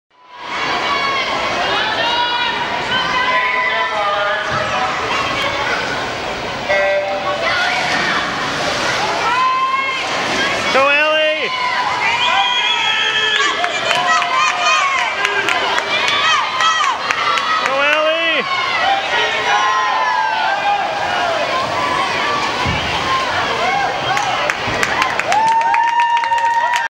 Ali 25 Free District Championship
Ali swimming 25 Free in the district championship meet.